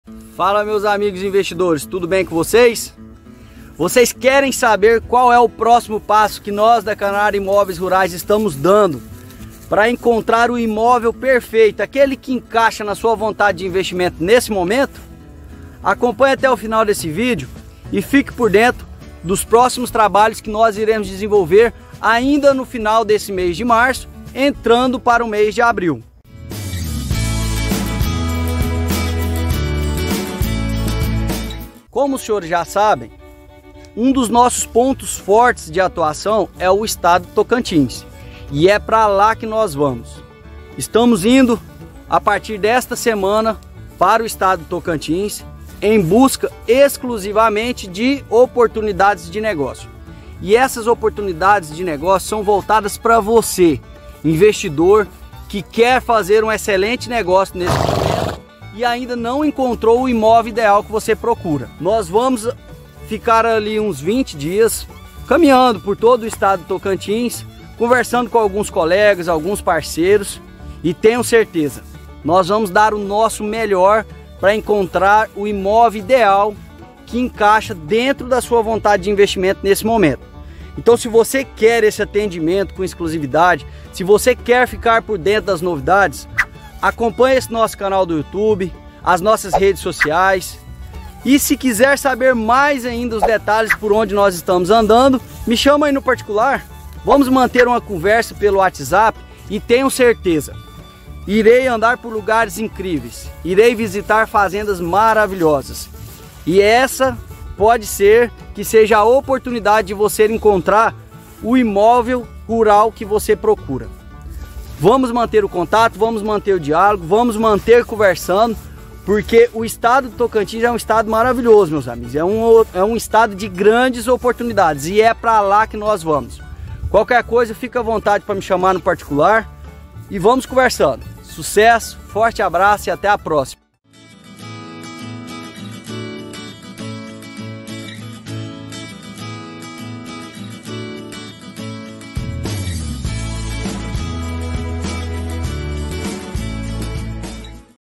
VAMOS EM BUSCA DAS MELHORES FAZENDAS A VENDA NO TOCANTINS?
Neste final do mês de Março retornaremos ao estado do Tocantins em busca das melhores fazendas à venda para te oferecer.

Se pretende fazer um investimento certo, seguro, comprando uma fazenda de qualidade, nós estamos prontos para te ajudar.

Acompanhem os nossos trabalhos e fique por dentro das novidades.

Qualquer demanda é só nos chamar no particular.

Sucesso a todos, forte abraço e até a próxima.